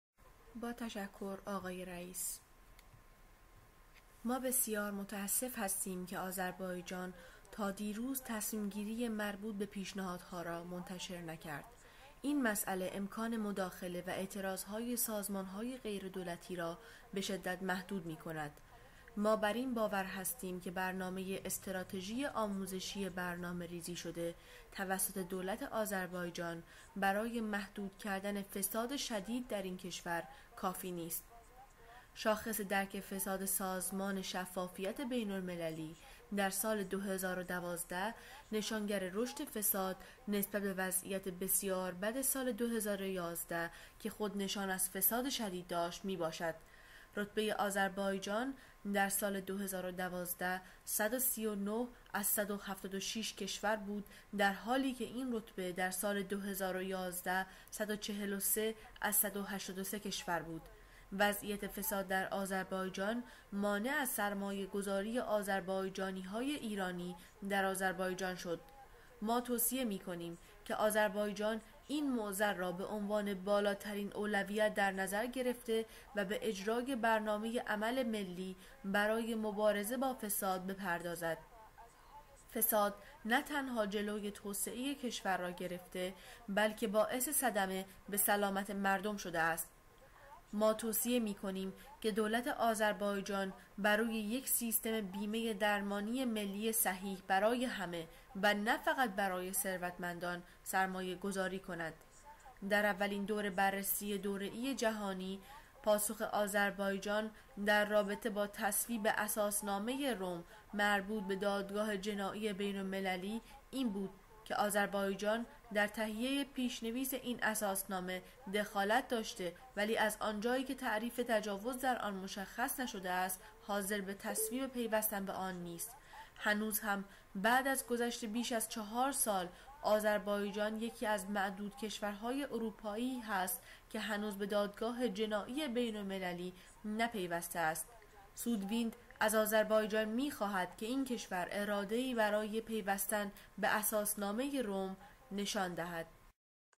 Sudwind, 24th Session HRC, UPR Azarbayjan , Farsi
موضعگیری سودویند در بیست و چهارمین نشست حقوق بشر سازمان ملل در خصوص وضعیت حقوق بشر در آذربایجان, ژنو , 19/09/2013 فارسی